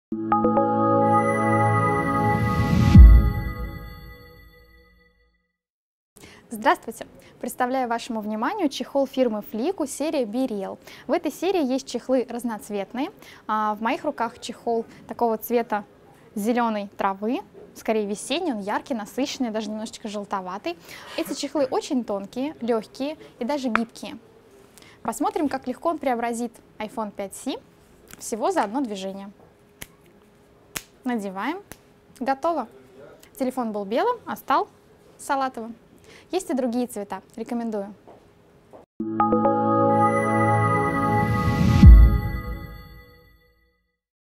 Чехол Fliku Ultra Slim Case зеленый
Очень тонкий чехол для iPhone 5c, с толщиной стенки всего 0,3 мм. Выполнен из прочного, эластичного материала с приятным на ощупь покрытием, и отлично защищает iPhone от царапин и мелких повреждений.